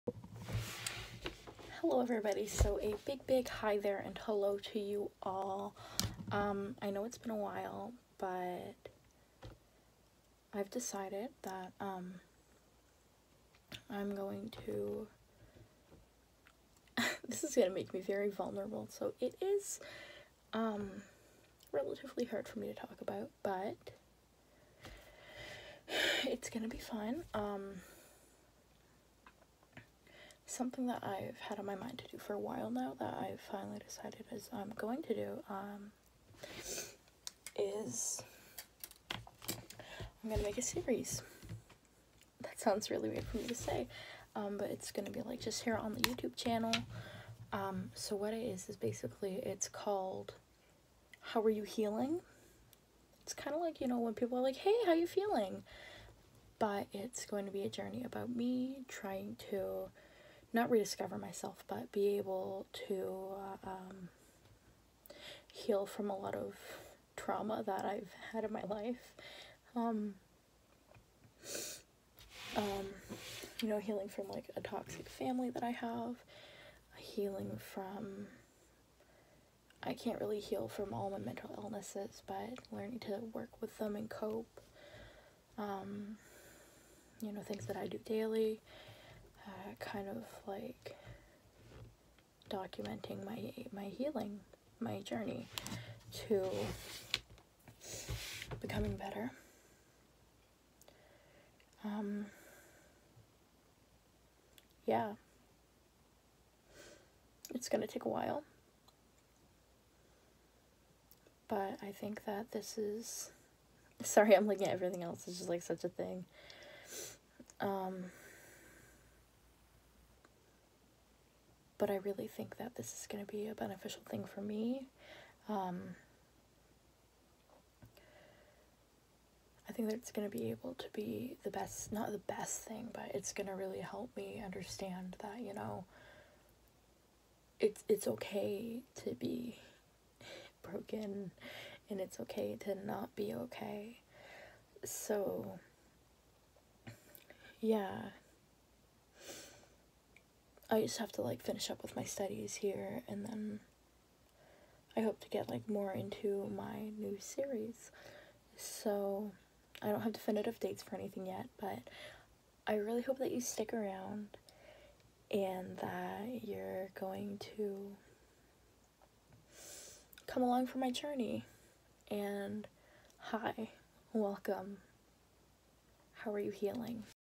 How Are You Healing?
Hello and welcome to a new series I am hoping to start later on this month. Follow along as I start my journey towards healing and discovering myself as an individual.

So stay a while. Grab a nice tea, your fave sweater and just hang out with me.

How AreYou Healing is my own idea and my own personalized method of healing from toxicity in my childhood home, moving out to my own place, coping in the midst of a pandemic with multiple mental health concerns, and a journey of rediscovery of who I am and hat I can become.